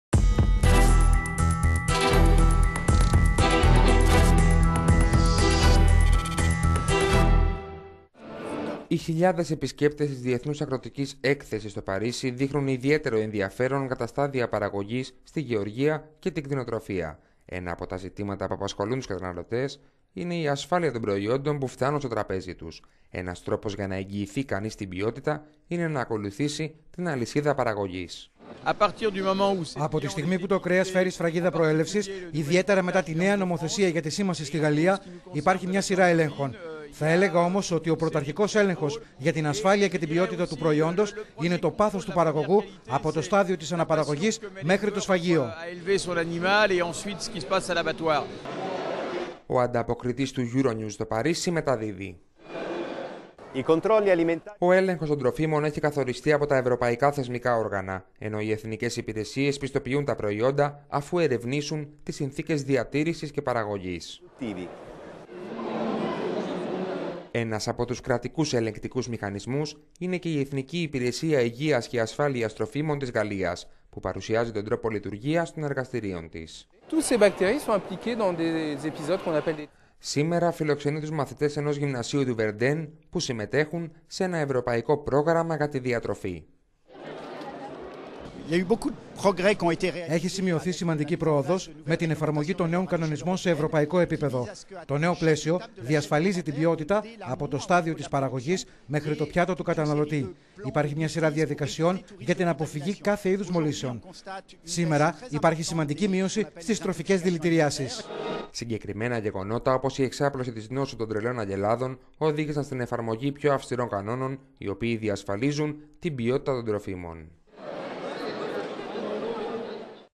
Παρίσι: Η ασφάλεια των τροφίμων στο επίκεντρο της Διεθνούς Αγροτικής Έκθεσης - focus
Η 51η Διεθνής Αγροτική Έκθεση στο Παρίσι άνοιξε και φέτος τις πύλες της. Οι εκατοντάδες χιλιάδες επισκέπτες δείχνουν ιδιαίτερο ενδιαφέρον για τα στάδια παραγωγής στη γεωργία και την κτηνοτροφία. Ένα από τα ζητήματα που απασχολούν τους καταναλωτές είναι η ασφάλεια των προϊόντων που φθάνουν στο τραπέζι μας. Ένας τρόπος για να εγγυηθεί κάνεις την ποιότητα είναι να ακολουθήσει την αλυσίδα παραγωγής.

«Από τη στιγμή που το κρέας φέρει σφραγίδα προέλευσης, ιδιαίτερα μετά τη νέα νομοθεσία για τη σήμαν...